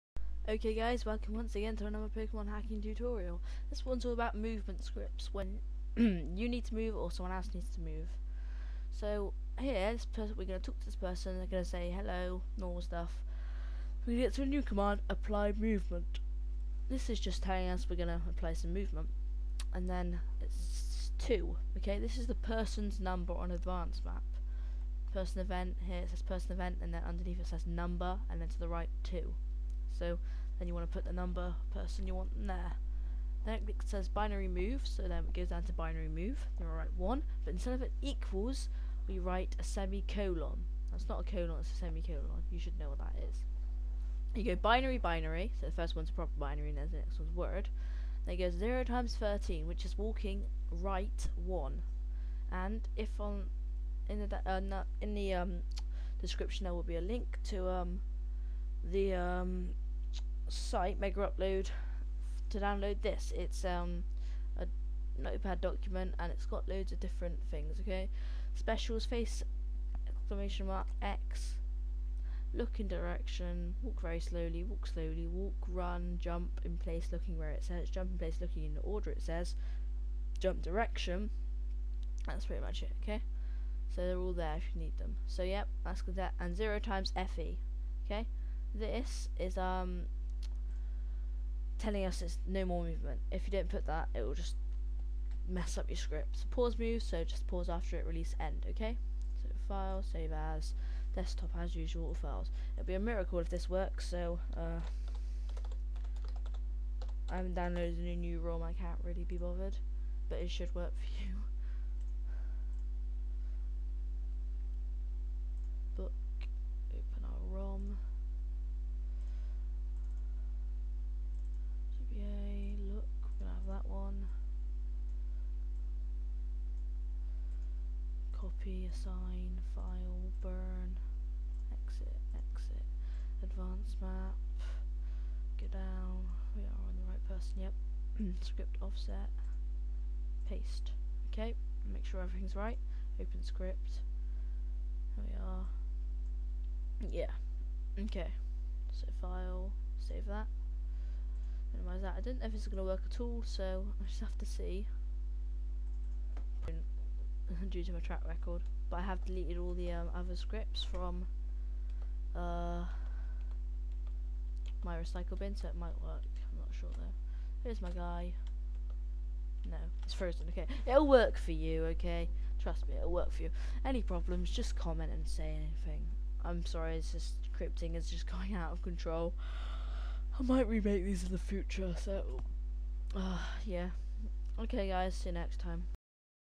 Pokemon Hacking Tutorials Part 16 - Scripting - Movement Scripts
How to move sprites during a script